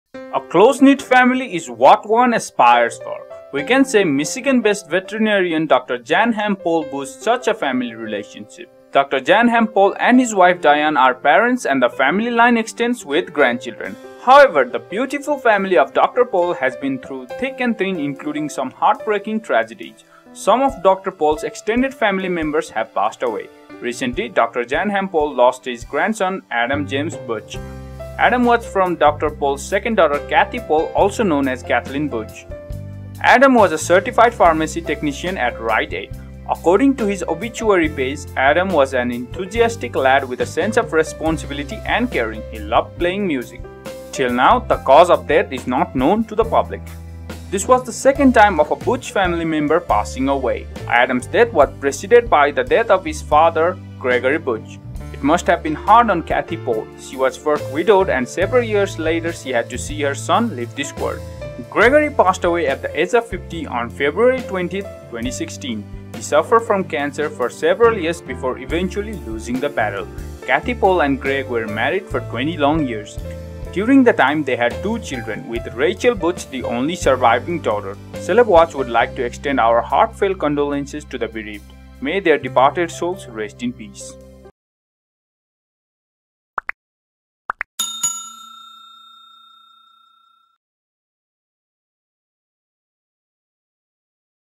Passed Away! The Incredible Dr. Pol family tragedy.
The Incredible Dr. Pol family tragedy. what happened to dr Jan Harm Pol's grandson? watch this video till the end to know more about the dr pol family who is not with him.
A close-knit family is what one aspires for.

We can say Michigan-based veterinarian Dr. Jan Ham Pol boosts such a family relation.

Dr. Jan Ham Pol and his wife Diane are parents and the family line extends with grandchildren.

However, the beautiful family of Dr. Pol has been through thick and thin including some heartbreaking tragedies.

Some of Dr. Pol's extended family members have passed away.

Recently Dr. Jan Ham Pol lost his grandson Adam James Butch.

Adam was from Dr.Pol’s second daughter Kathy Pol also known as Kathlene Butch.

Adam took his last breath at the age of 23 on September 18, 2019.

Adam was a Certified Pharmacy Technician at Rite Aid.

According to his obituary page,
Adam was an enthusiastic lad with a sense of responsibility and caring, he loved playing music.

Till now his cause of death is not known to the public.

This was the second time the member passed away in the Butch family.

Adam’s death was preceded by the death by his father Gregory butch.

It must have been hard on Kathy Pol, she was first widowed then several years later she had to see her son leave this world.

Gregory passed away at the age of 50 on February 20, 2016.

He suffered from cancer for several years before eventually losing the battle.

Kathy Pol and Greg were married for twenty long years.

During the time they had two children with Rachel Butch the only surviving daughter.

Celebwatch would like to extend our condolences to the bereaved. May the departed souls rest in peace.